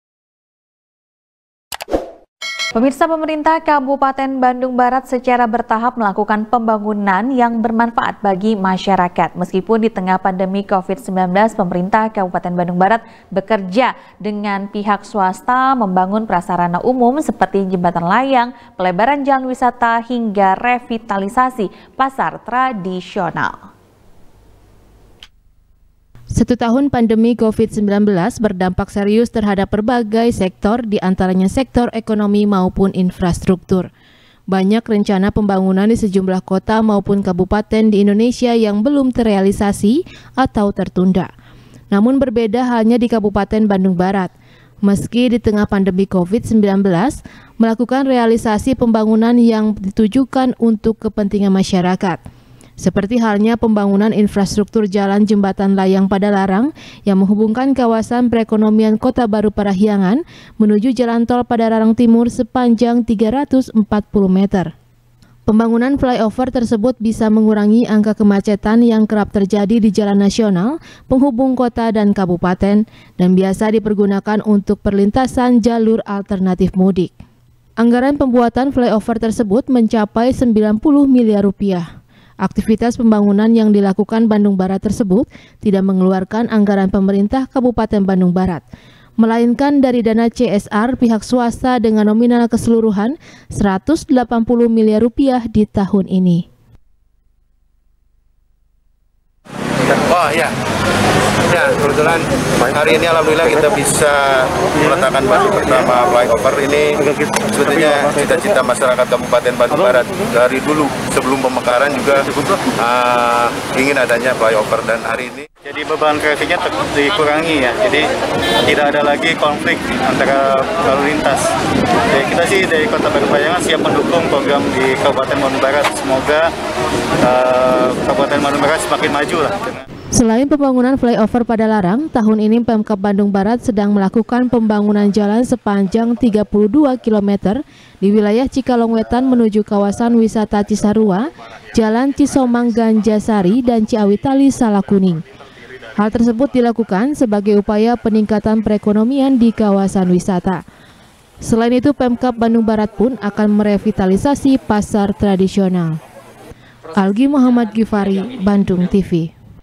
Pembangunan Fly Over Padalarang Tanpa Dana Pemerintah | SBR | BANDUNGTV
SBR | BANDUNGTV | KRISANTI | Pemerintah Kabupaten Bandung Barat, secara bertahap melakukan pembangunan yang bermanfaat bagi masyarakat, meskipun ditengah pandemi Covid 19. Pemerintah kabupaten bandung barat bekerja sama dengan pihak swasta, membangun prasarana umum seperti jembatan layang, pelebaran jalan wisata, hingga revitalisasi pasar tradisional.